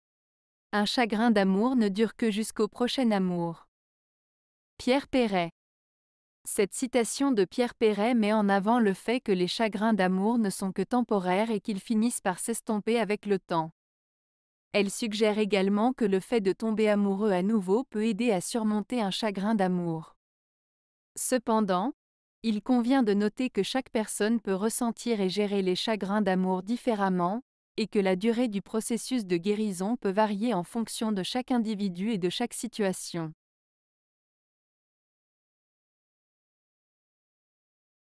Un chagrin d'amour ne dure Que jusqu'au prochain amour. - Pierre Perret
Cette citation de Pierre Perret met en avant le fait que les chagrins d'amour ne sont que temporaires et qu'ils finissent par s'estomper avec le temps. Elle suggère également que le fait de tomber amoureux à nouveau peut aider à surmonter un chagrin d'amour. Cependant, il convient de noter que chaque personne peut ressentir et gérer les chagrins d'amour différemment, et que la durée du processus de guérison peut varier en fonction de chaque individu et de chaque situation.